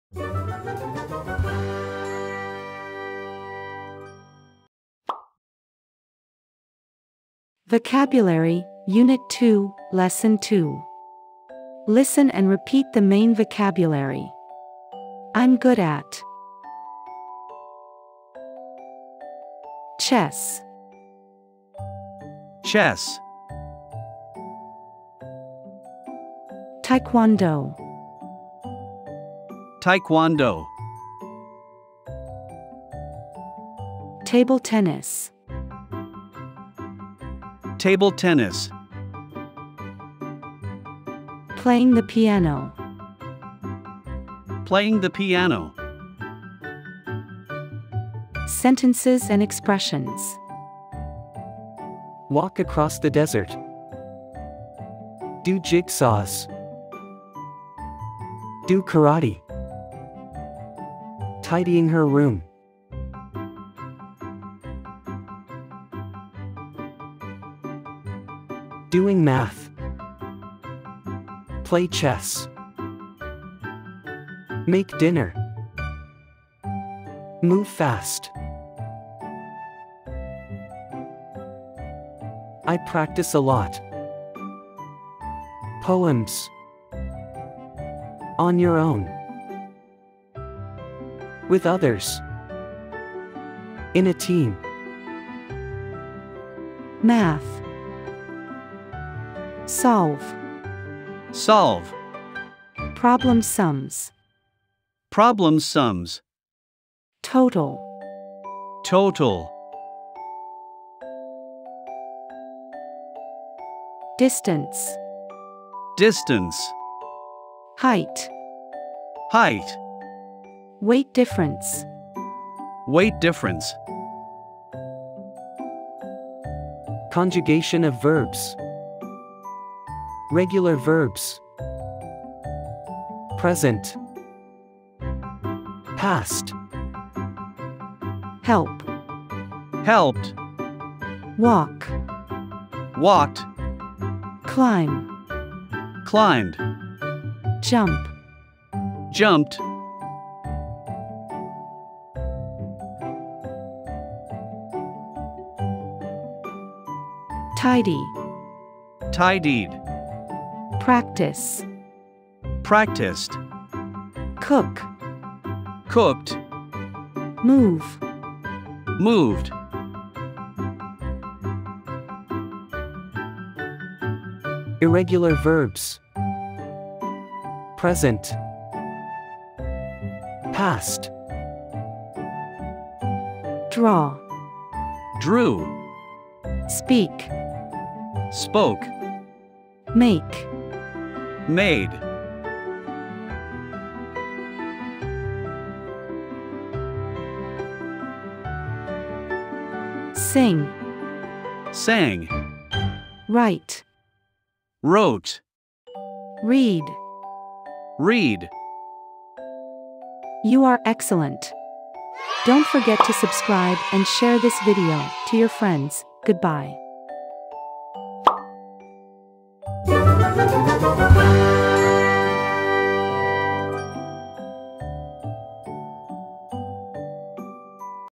connect primary 5 unit 2 lesson 2 كونكت خامسه ابتدائي الوحدة الثانيه الدرس الثانى
Vocabulary, Unit 2, Lesson 2
Listen and repeat the main vocabulary
I'm good at
chess
chess
taekwondo
taekwondo
table tennis
table tennis
playing the piano
playing the piano
Sentences and Expressions
walk across the desert
do jigsaws
do karate
tidying her room
doing math
play chess
make dinner
move fast
I practice a lot.
poems
on your own
with others
in a team
Math
solve
solve
problems-sums
problems-sums
total
total
distance
distance
height
height
 weight difference
 weight difference
Conjugation of verbs
Regular verbs
Present
Past
help
helped
walk
walked
climb
climbed
jump
 jumped
tidy
tidied
practice
practiced
Cook
cooked
move
moved
Irregular verbs
Present
Past
draw
drew
speak
spoke
make
made
sing
sang
write
wrote
read
read